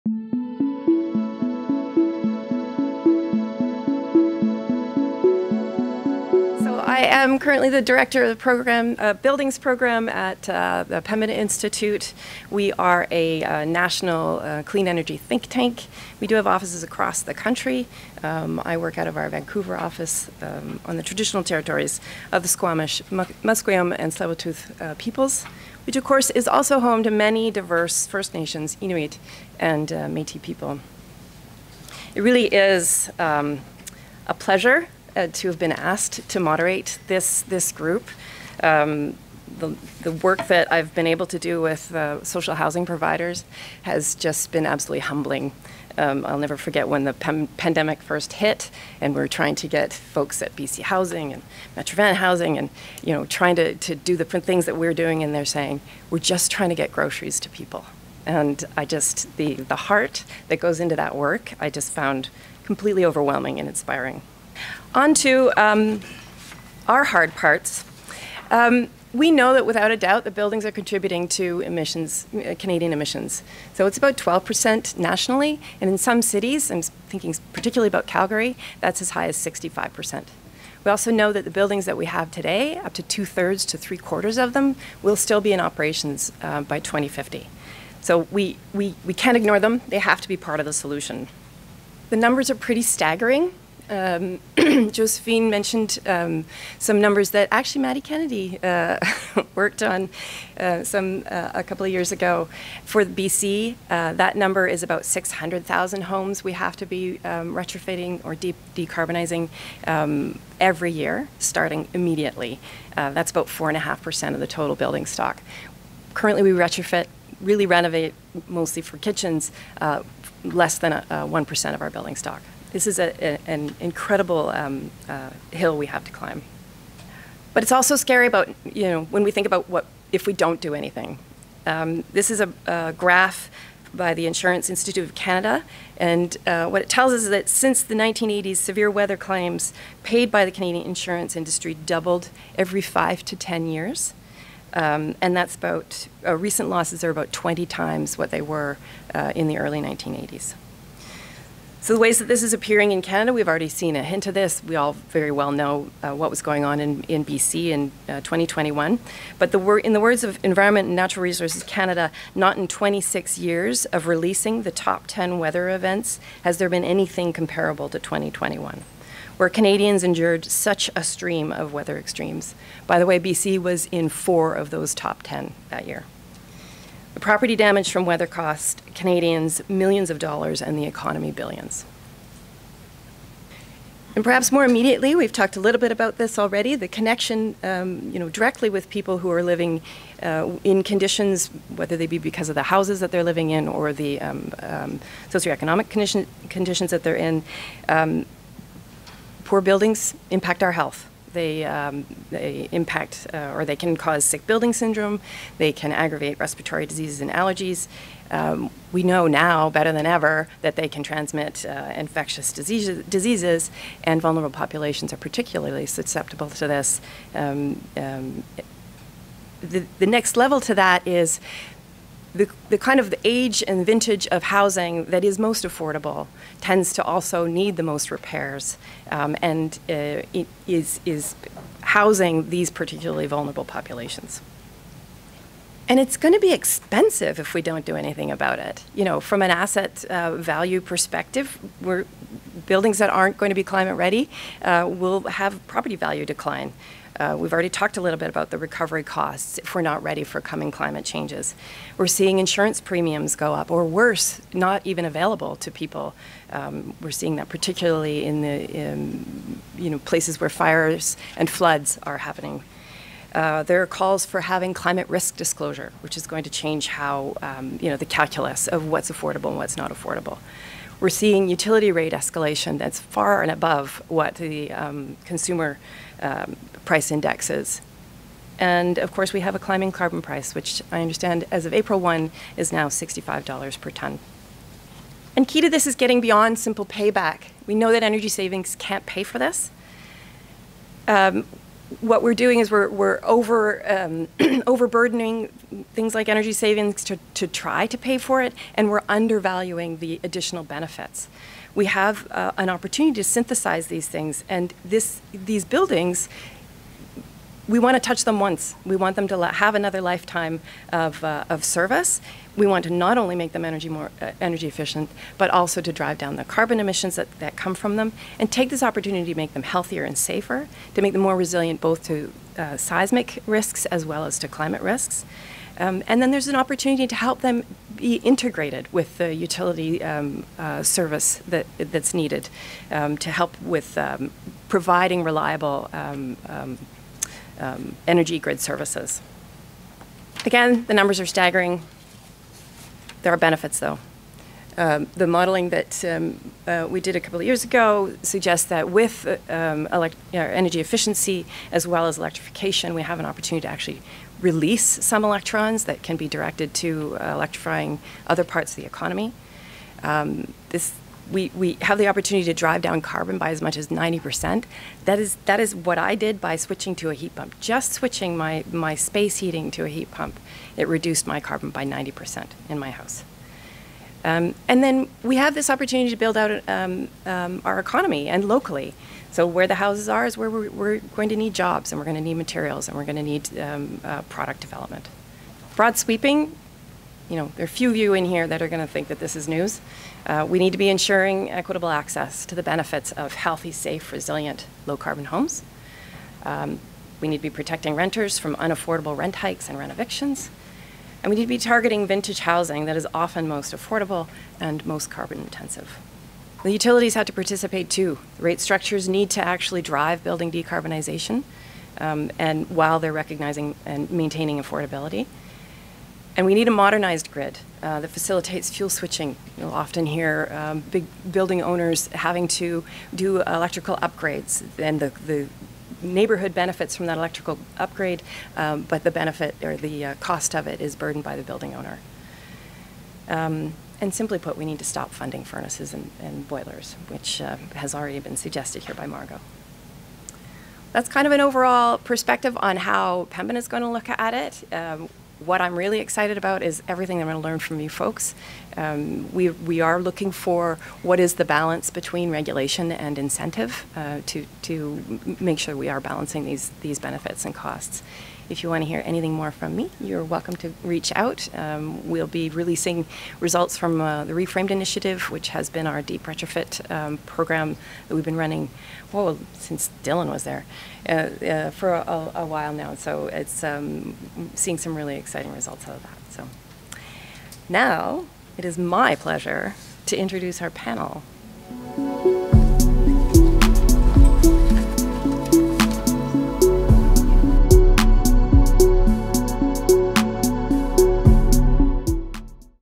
Energy Justice Forum — Betsy Agar, Session 2: Pathways to Home Energy Justice
Betsy Agar, Buildings Director, Pembina Institute, speaks at the Energy Justice Forum UBC Robson Square on xʷməθkwəy̓əm (Musqueam), Skwxwú7mesh (Squamish), and səlilwətaɬ (Tsleil-Waututh) territories, in Vancouver. The forum was held on April 12, 2023.